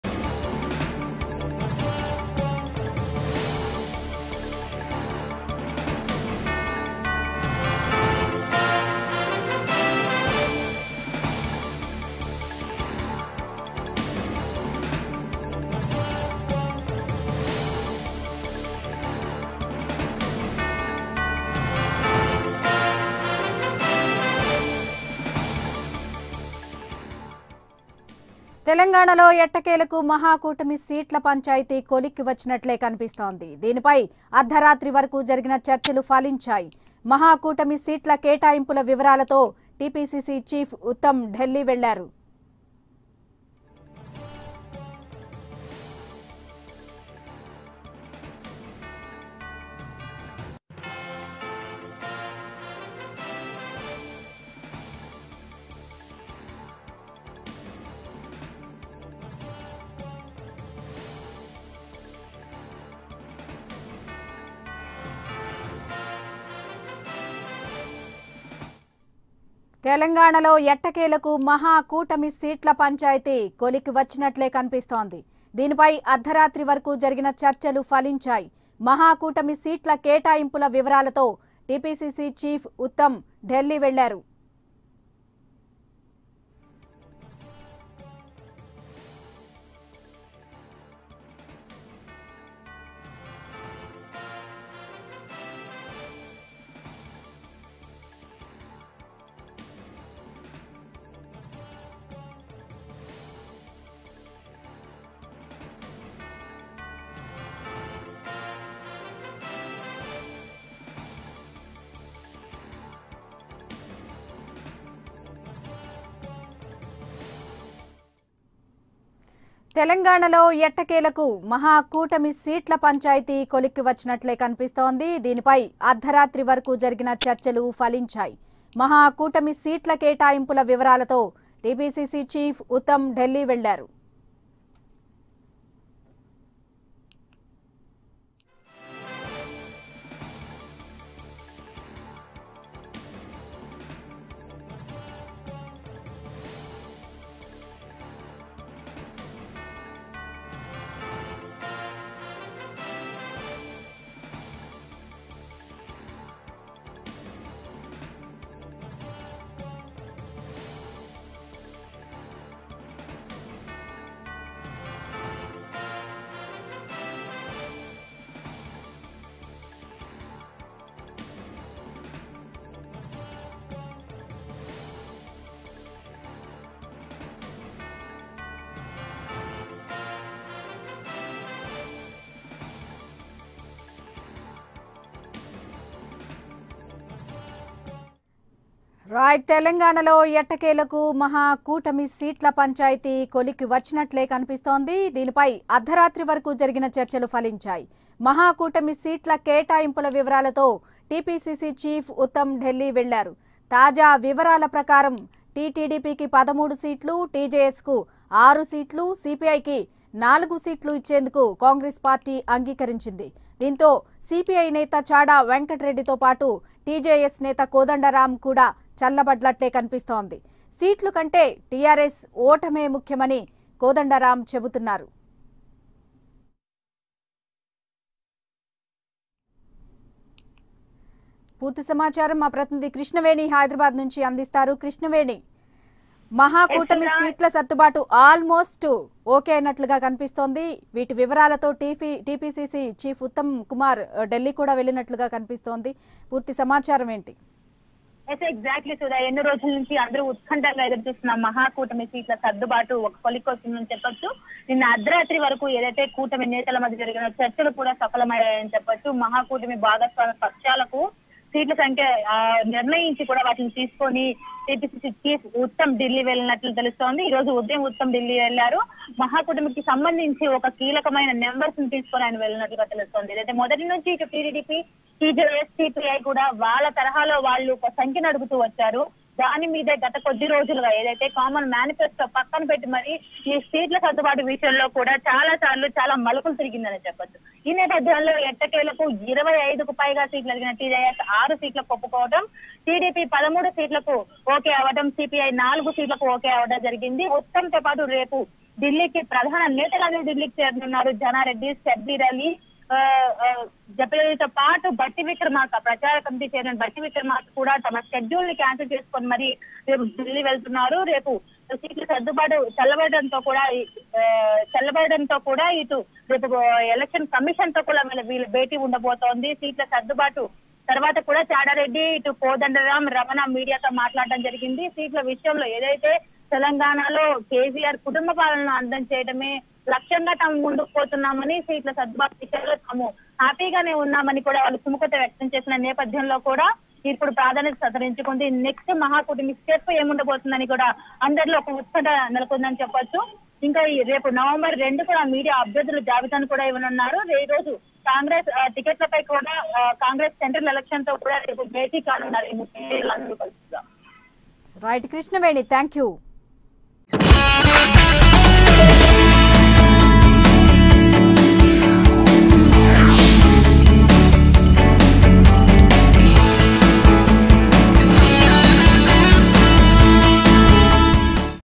PCC Chief Uttam Kumar To Meet Rahul Gandhi Over Mahakutami Seats Issue | AP24x7
PCC Chief Uttam Kumar to meet Rahul Gandhi over Mahakutami seats Issue. For TDP 13 seats, for TJS 6 seats, for CPI 4 seats are allocated. Leaders Janareddy and Shabbir Ali also went to Delhi along with PCC Chief Uttam Kumar. All positive vibes are going on among grand alliance parties.